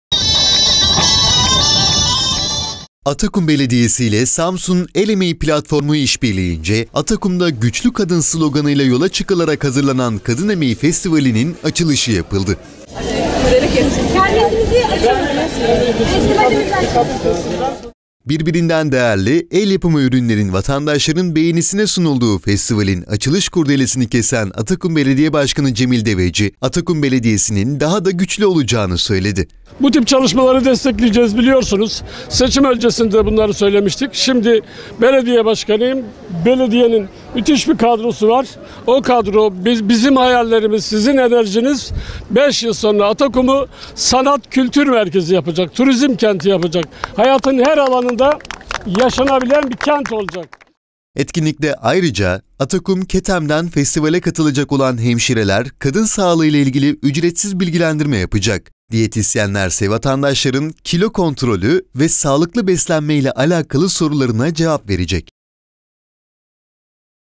Atakum'da "Kadın Emeği Festivali" başladı!
Birbirinden değerli el yapımı ürünlerin vatandaşların beğenisine sunulduğu festivalin açılış kurdelesini kesen Atakum Belediye Başkanı Cemil Deveci, Atakum Belediyesi’nin daha da güçlü olacağını söyledi. Böyle çalışmaları destekleyeceklerini ifade eden Deveci, şu ifadelere yer verdi.

Etkinlikte ayrıca Atakum KETEM'den festivale katılacak olan hemşireler kadın sağlığıyla ilgili ücretsiz bilgilendirme yapacak. Diyetisyenler ise vatandaşların kilo kontrolü ve sağlıklı beslenmeyle alakalı sorularına cevap verecek